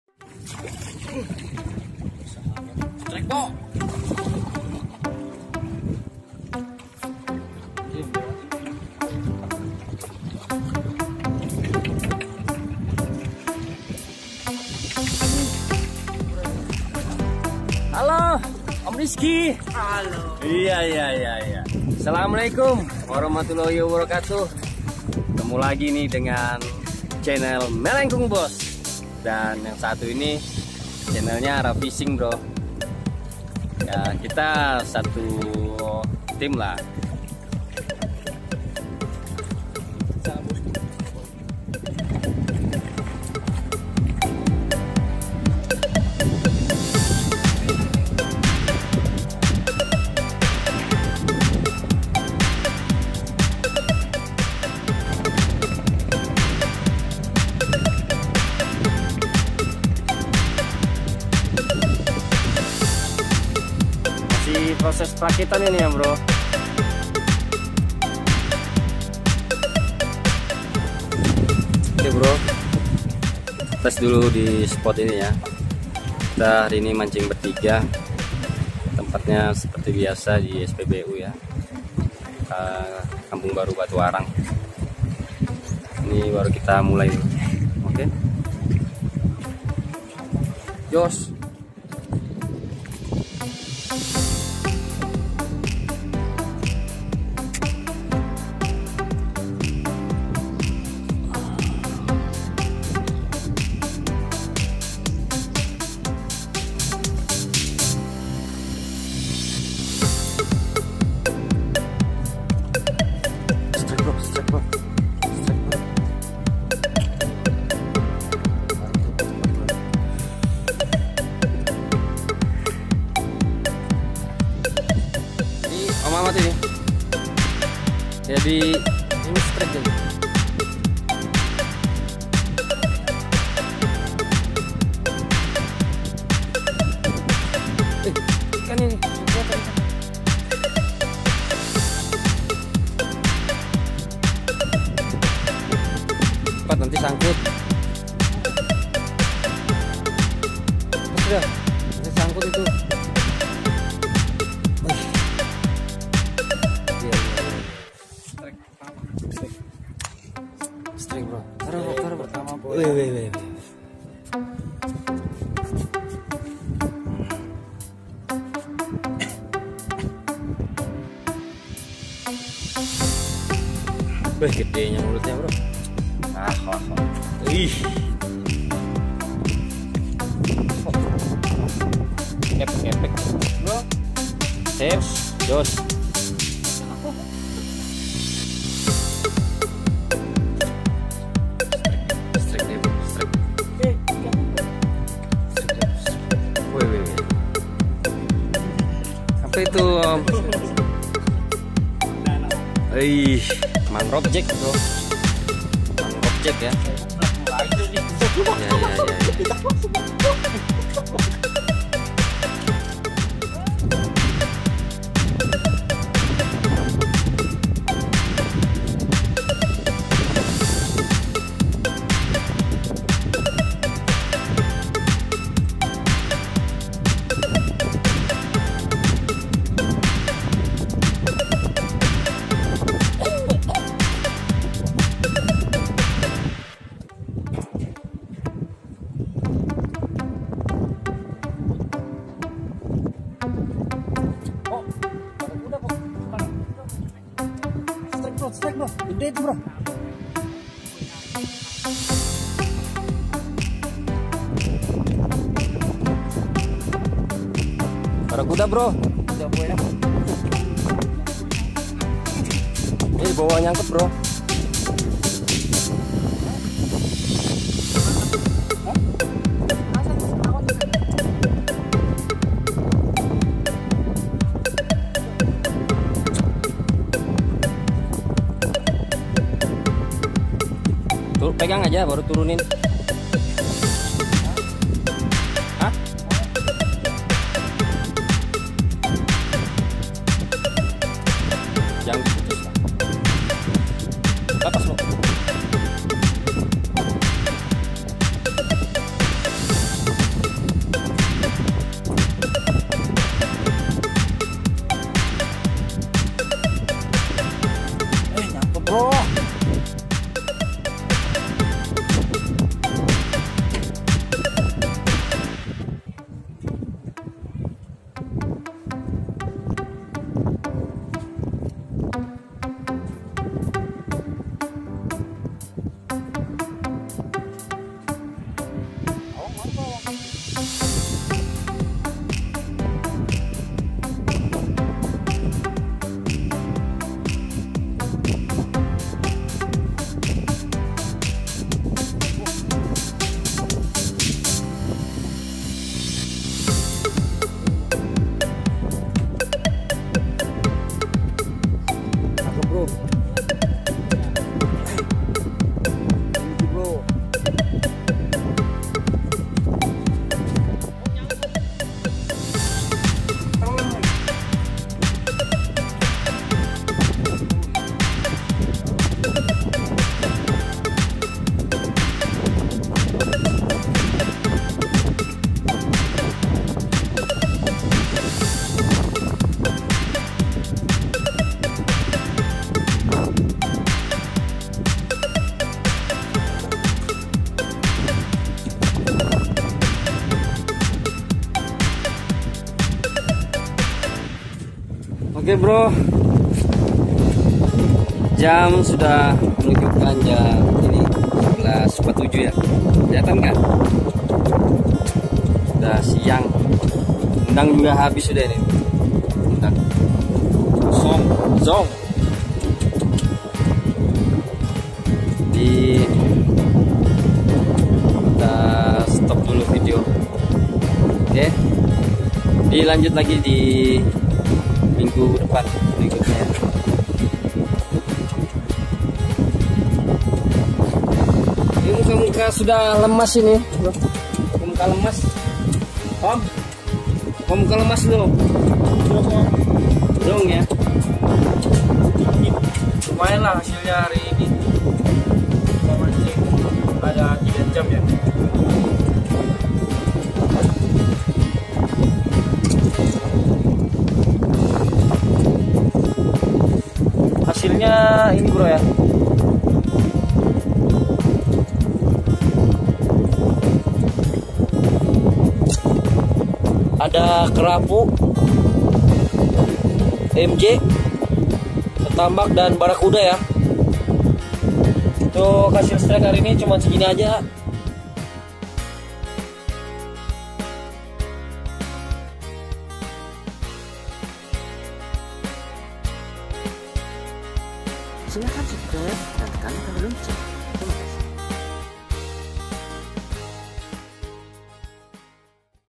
Siapa sangka mancing di kolong ternyata banyak ikan bermain
Balikpapan 01/09/2019
Kalimantan timur

Assallamuallaikum.
Hasil tarikan hari ini ikan barakuda, kerapu, mangrov jack, ketambak, teknik timah tipis di atas cocok utk mancing di arus yg sedang.

Tanpa kalian kami bisa apa.

Salam amis.

Terima kasih.